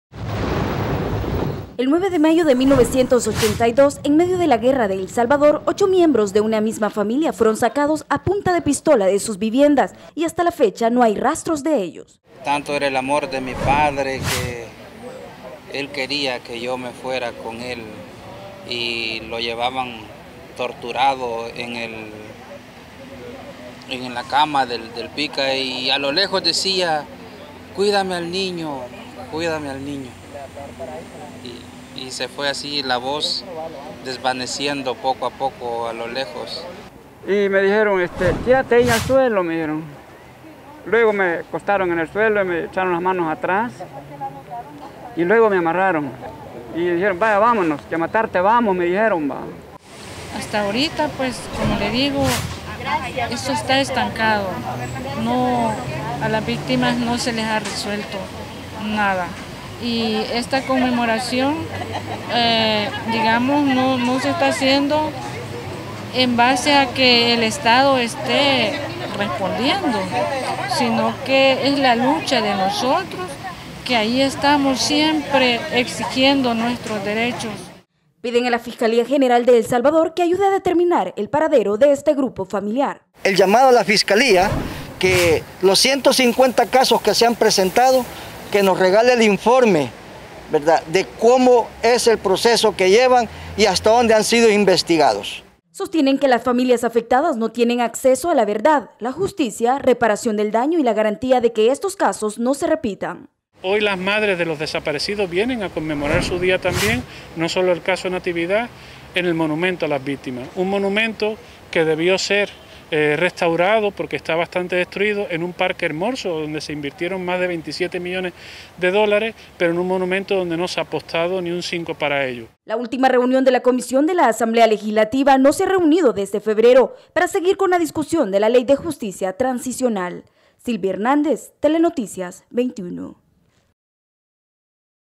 Familiares de desaparecidos durante el conflicto armado recordaron a sus seres queridos
El monumento a La Memoria y la Verdad, ubicado en el Parque Cuscatlán, fue el escenario donde familiares de desaparecidos durante el conflicto armado recordaron a sus seres queridos.